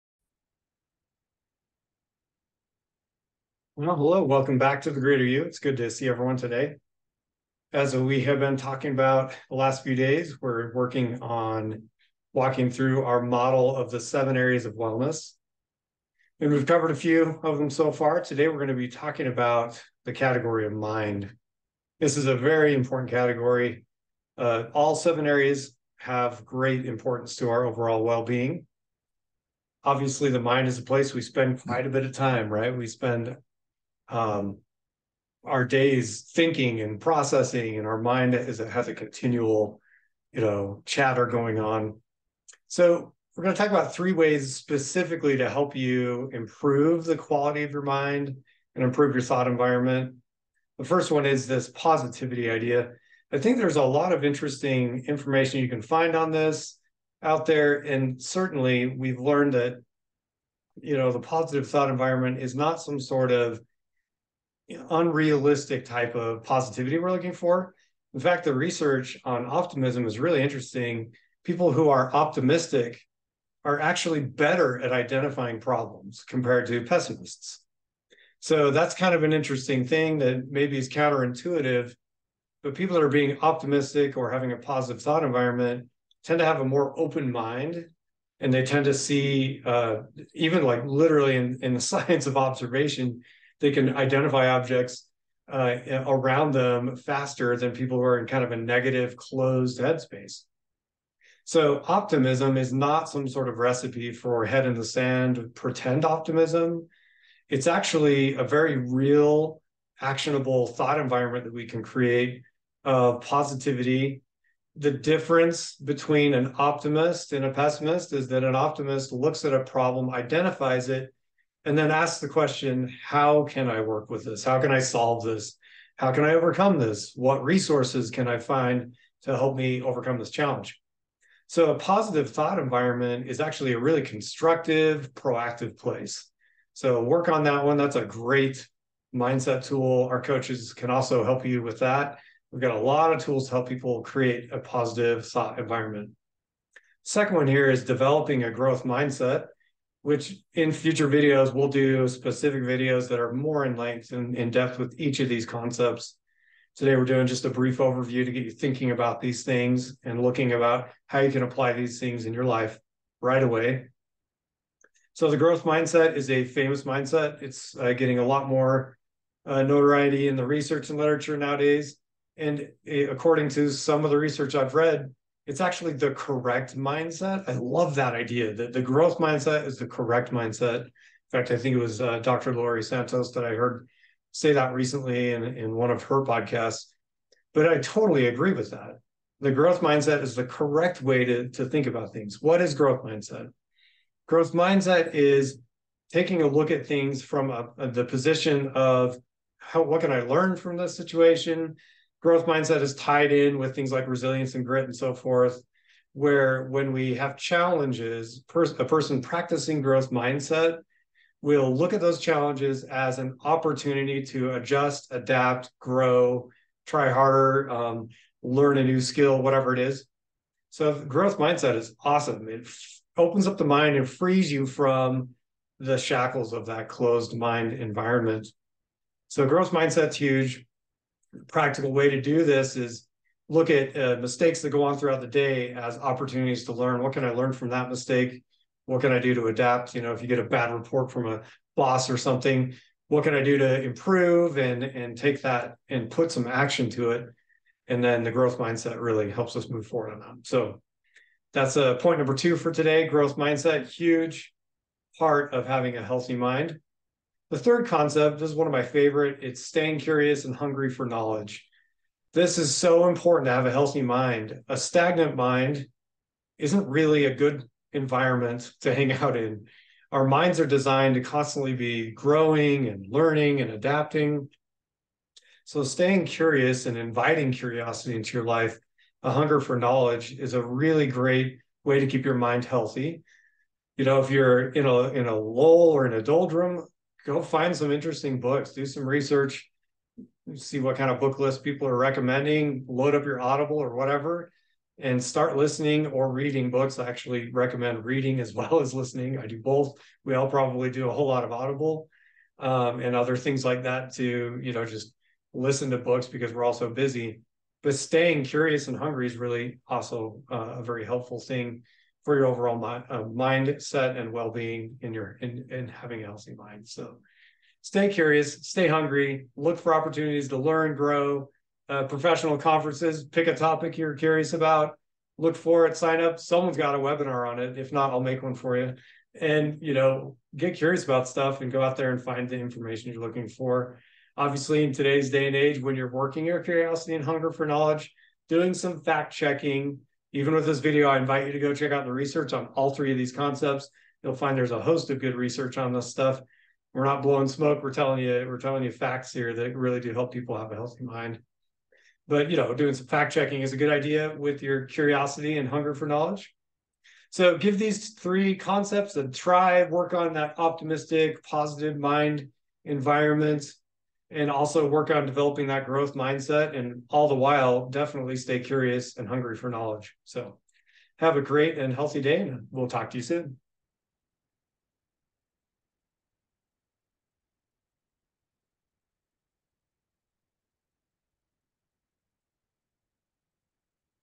#3 HEALTHY MIND: 7 Areas of Well-Being
We discuss the importance of the mind in our overall well-being and share three ways to improve the quality of our thoughts. I talk about the power of positivity, the benefits of a growth mindset, and the importance of staying curious and hungry for knowledge. Join me as we explore these concepts and learn how to create a positive thought environment for a healthier mind.

00:00 Introduction
00:41 Positivity
02:25 Growth Mindset
04:17 Curiosity and Hunger